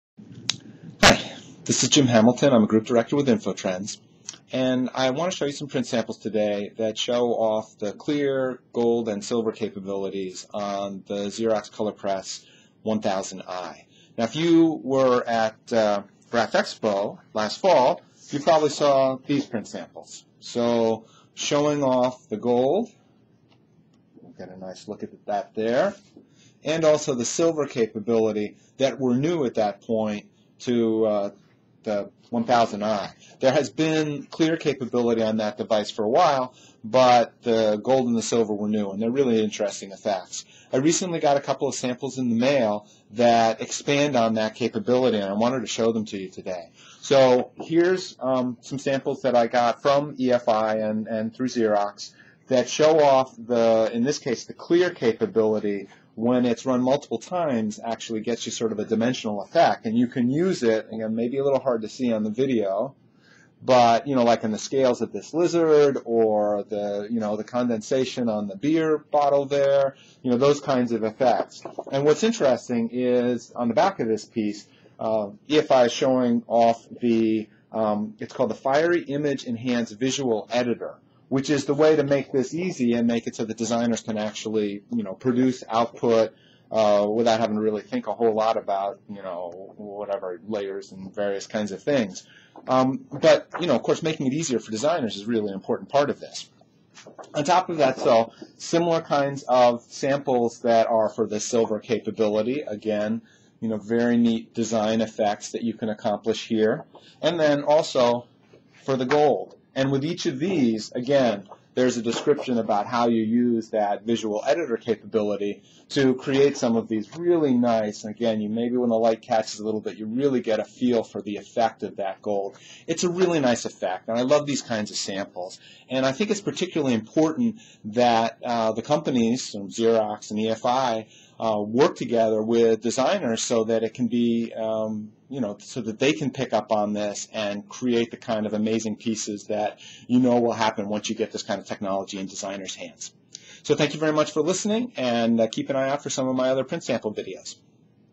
Silver, Gold, and Clear Print Samples from the Xerox 1000i
Jim Hamilton, a Group Director at InfoTrends, discusses some samples showing the use of gold, silver, and multi-pass clear using the Xerox Color Press 1000i and its EFI Fiery digital front end system with Fiery Image Enhance Visual Editor.